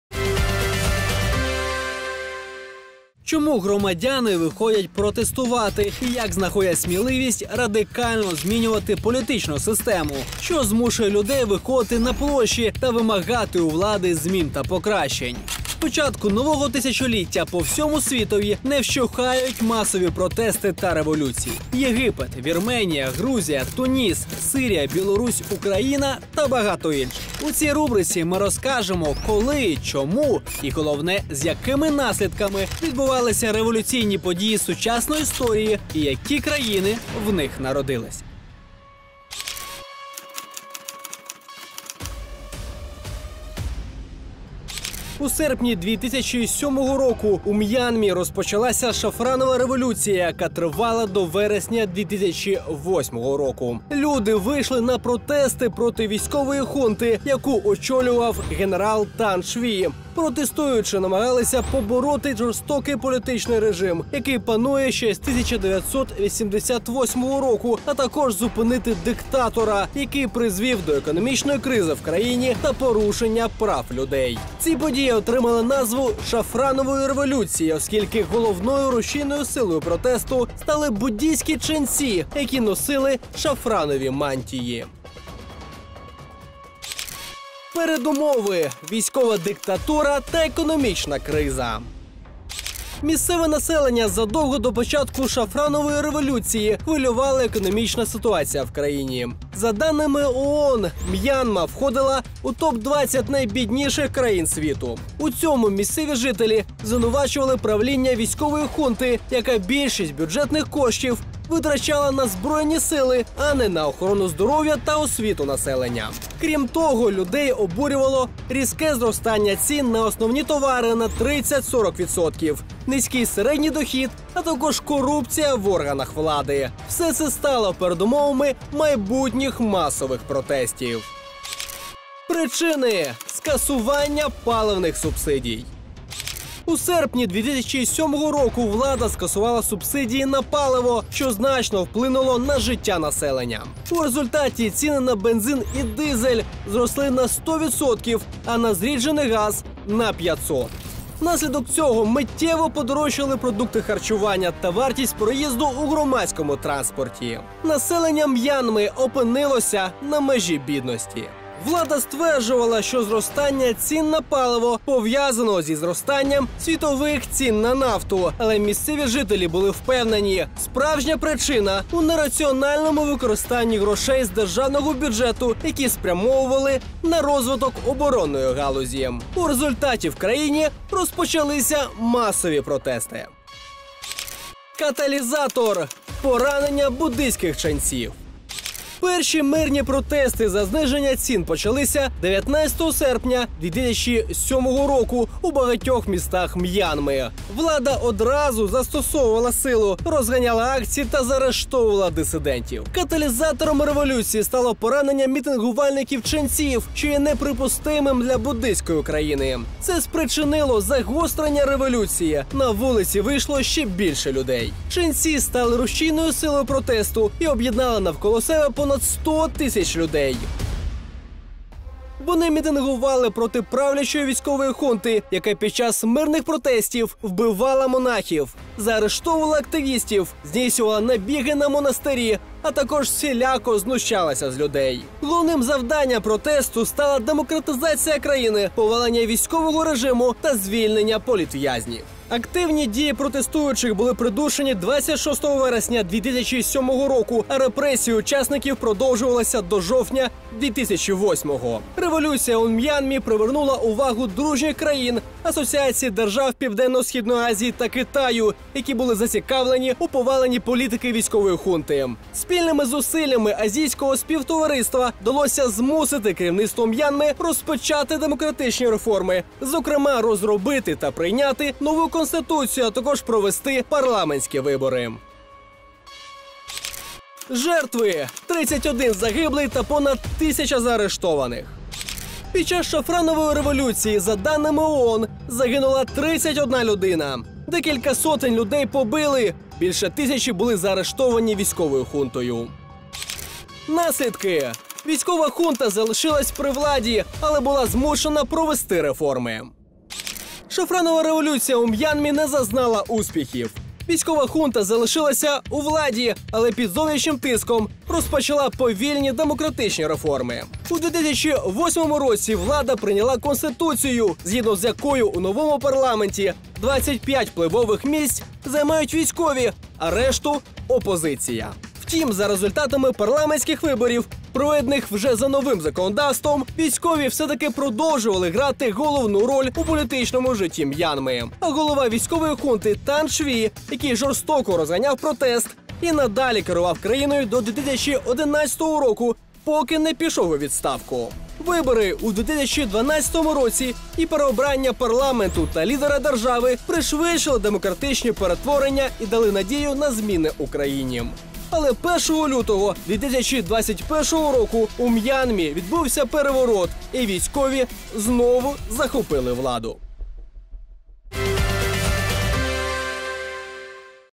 Шафранова революціяу М'янмі, 2007
Головною рушійною силою масових акцій протесту під час Шафранової революції у М'янмі стали буддійські ченці. Вони разом з тисячами жителів цієї країни вийшли проти правління військової хунти та диктатора. Учасників демонстрацій намагалися побороти жорстокий політичний режим і зупинити економічну кризу та порушення прав людей. Чи вдалося жителям М'янми домогтися свого та змінити ситуацію в країні? Більше про ці події розкажемо у рубриці «Народжені революцією».